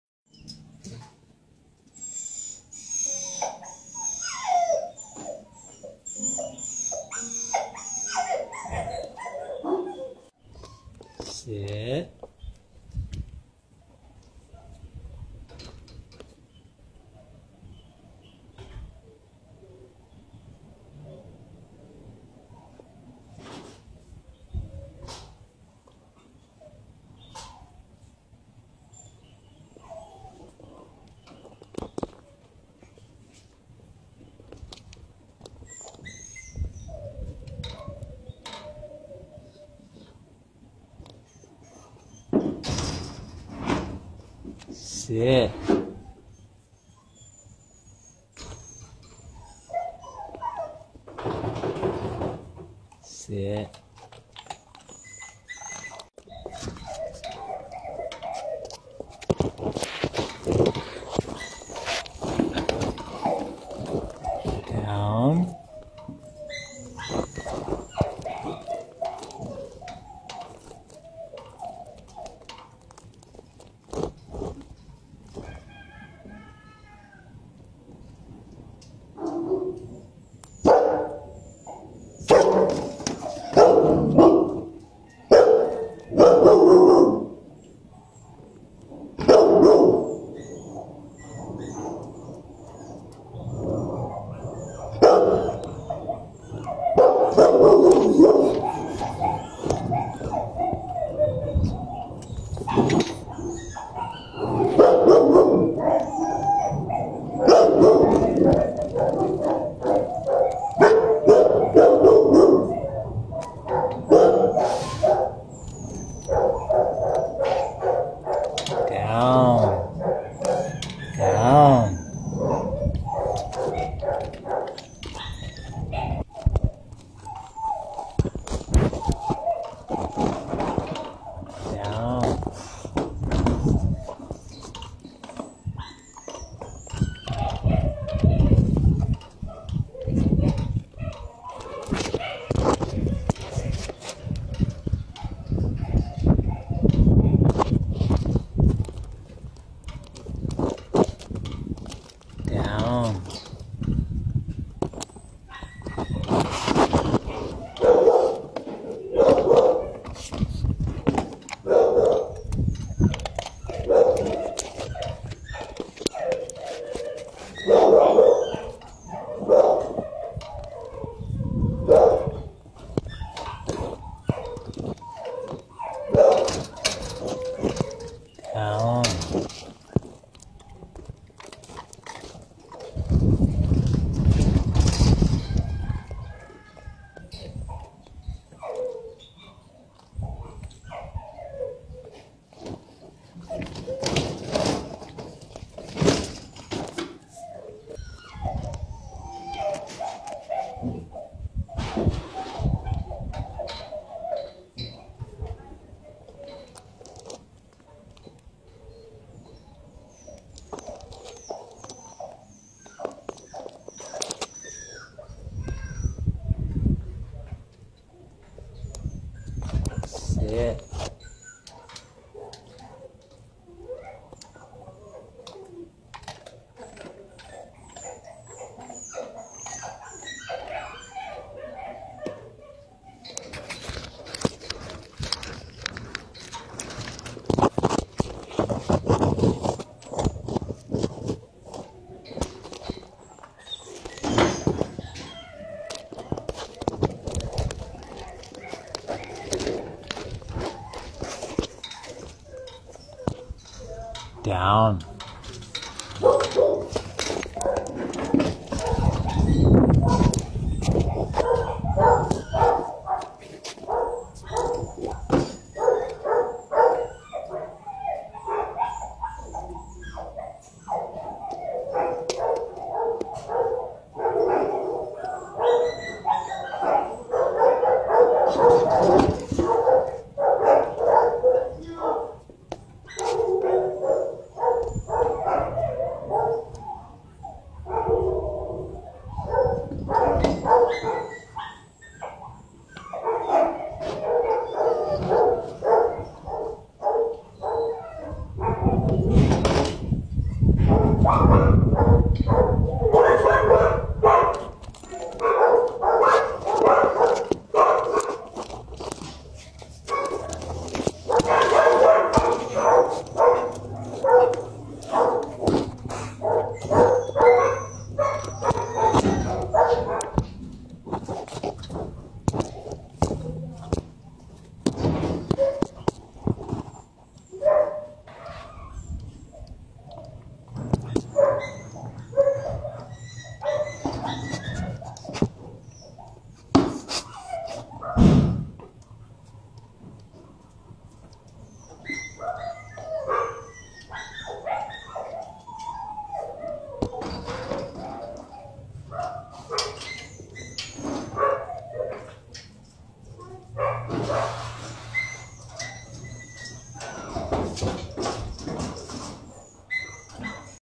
Old film❤️Ethel 1y Pet Id 840418 lovin Humane Society of Southern Arizona❤️3-28-2017adopted3-28-2017
❤️Ethel 1y Pet Id 840418 Shepherd. She's Teaching me how She can Sit & Lay Down if asked correctly.
P.S. On the last one She sat but I didn't ask Her the correct way that she understands, so she didn't Lay Down for me all the way. It's all good😊